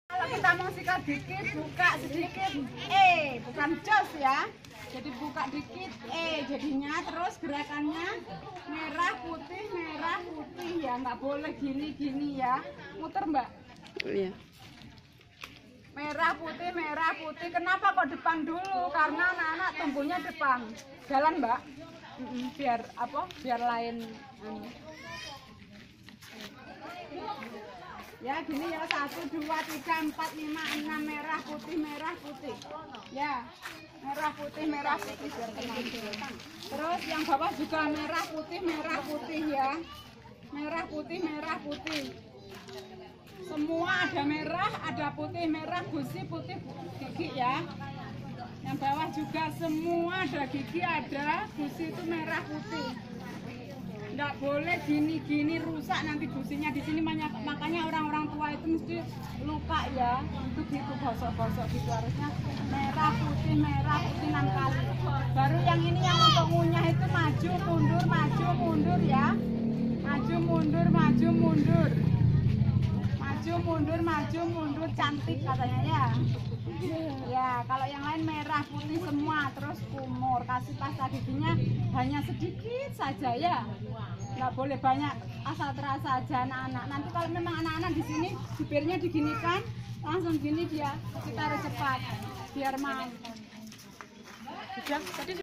DHE(Dental Health Education) 2016 Posyandu Srikandi Siliragung
for cild and mother,  E. H. Ermiana dentist,  pdgi Banyuwangi